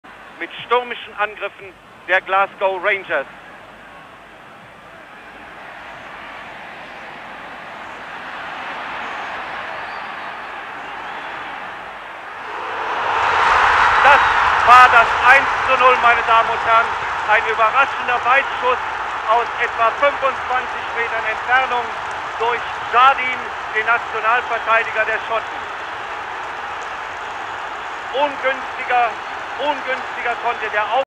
Sandy Jardine Goal v Bayern Munich
Only took a minute for Sandy to send us on our way to Barcelona